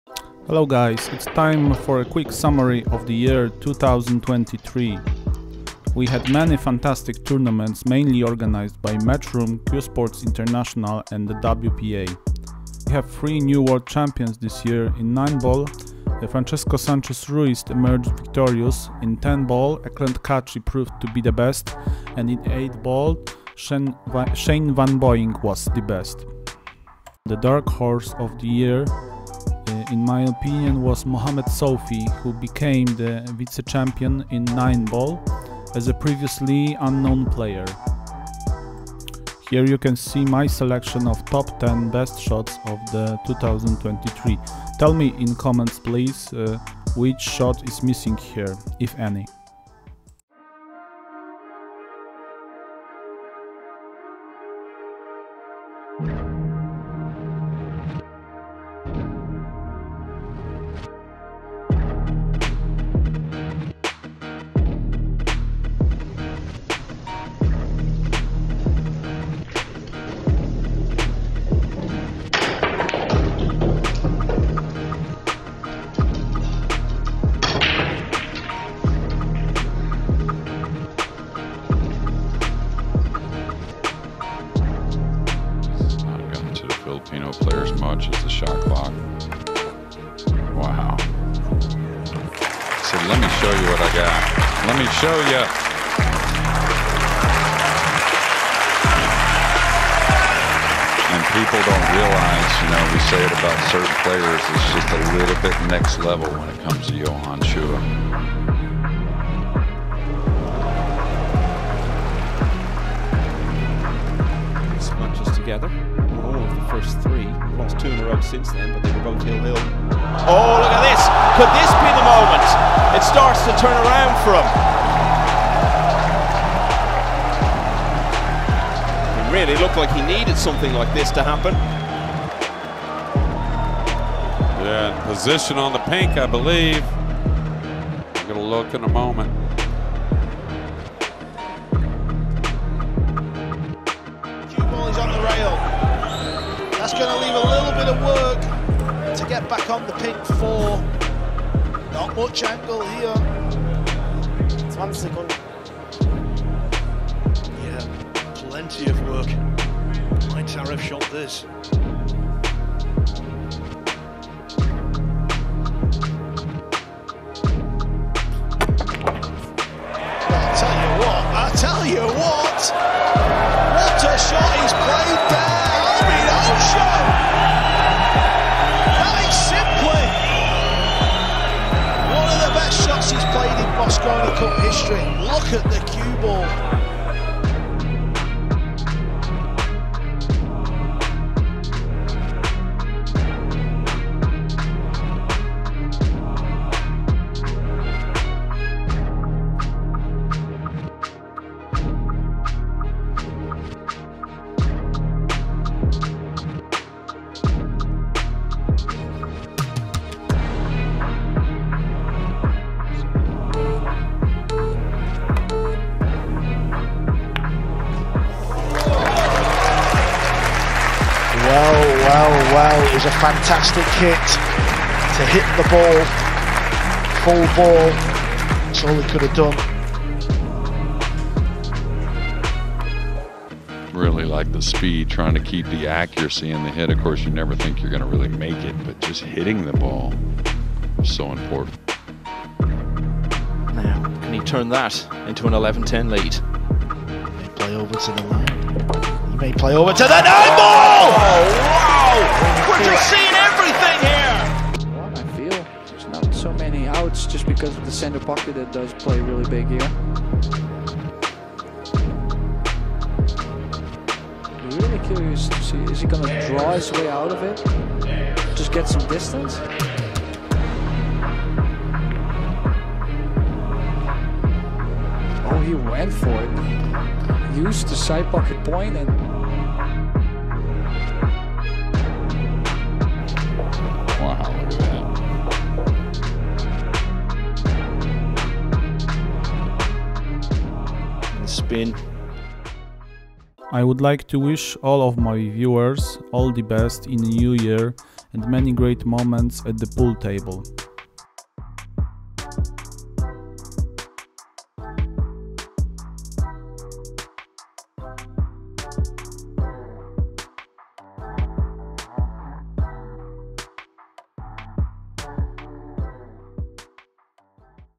The RIGOROUS selection of TOP 10 Shots of 2023 | Happy New Year to all PoolLovers
Cracow's pool fan/player publishes best-of shots with some background music and sometimes the commentary vlogs about professional pool also.
Cheers guys ! Have a nice watch.